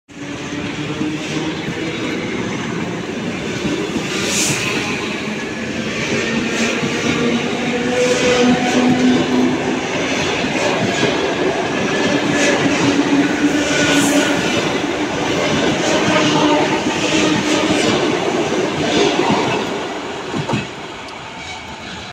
Электропоезд ЭД4М-0285 Отправляется со станции Орехово-Зуево
Electric train ED4M-0285 Departs from Orekhovo-Zuevo station